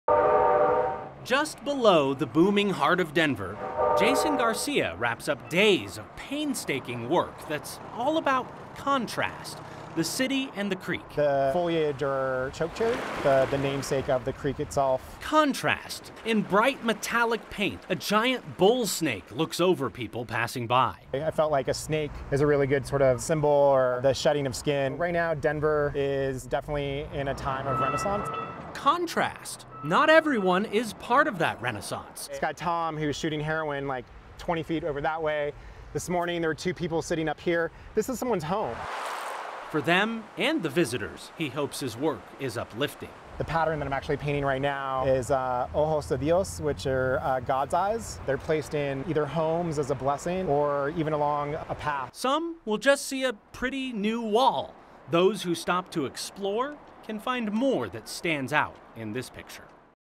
Snake mural captures contrast of Denver
Just below the booming heart of Denver, Jason Garcia wraps up days of painstaking work that's all about contrast.